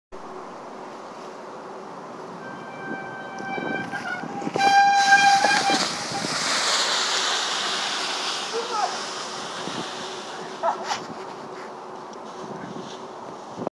A "Controlled fall"
My brother decided to take on a steep drop in when in the woods and after hitting the leaves on the ground started weaving before gently taking a lay down or as he said "it was a controlled fall" Sent from my Windows Phone